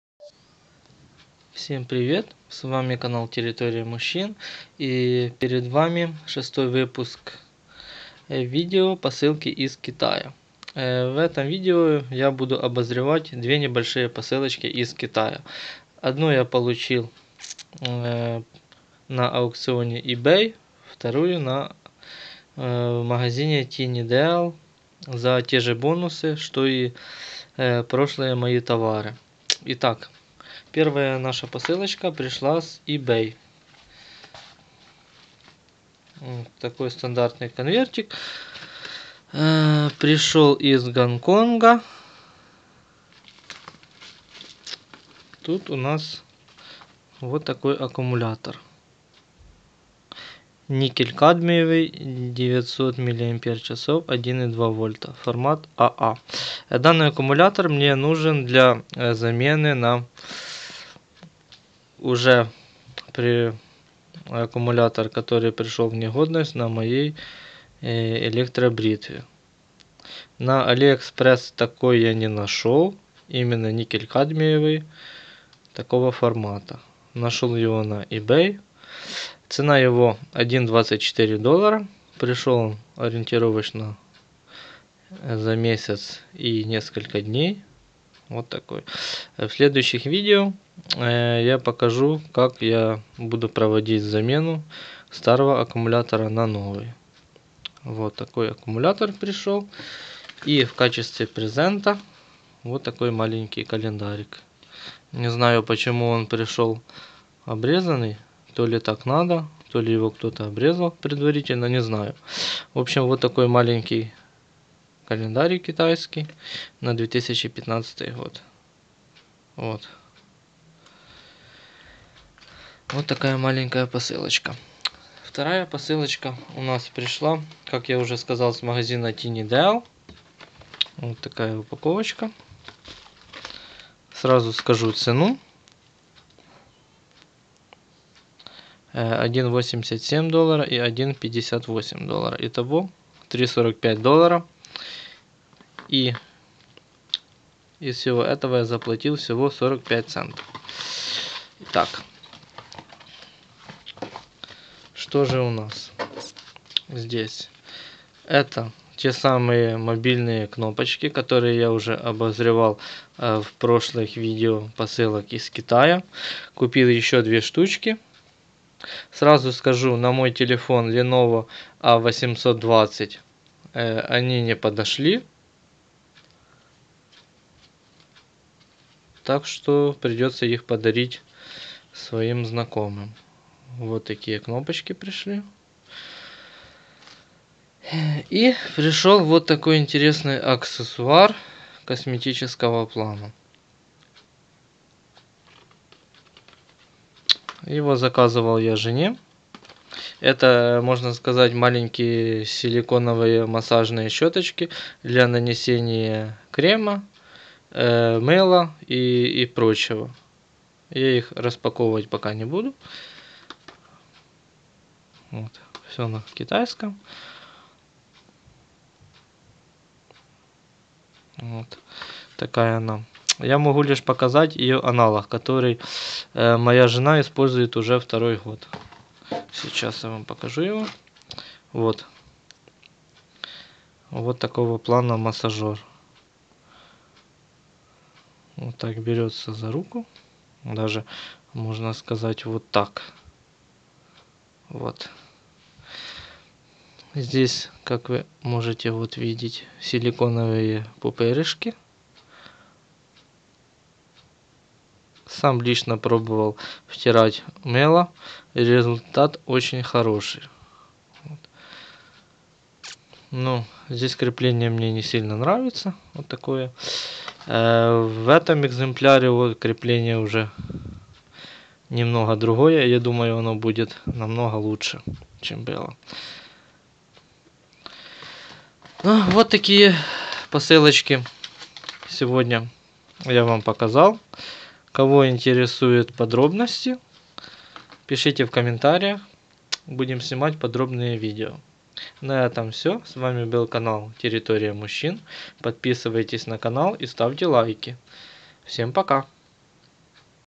Обзор посылок из Китая. Часть 6
В очередном обзоре посылок из Китая Вы уведите аккумулятор AA Ni-Cd 900 mAh 1.2V купленный на Ebay в первой посылке. Во второй посылке у нас уже знакомая по предыдущему видео программируемая кнопка для телефона и силиконовые щеточки для умывания. Подписываемся!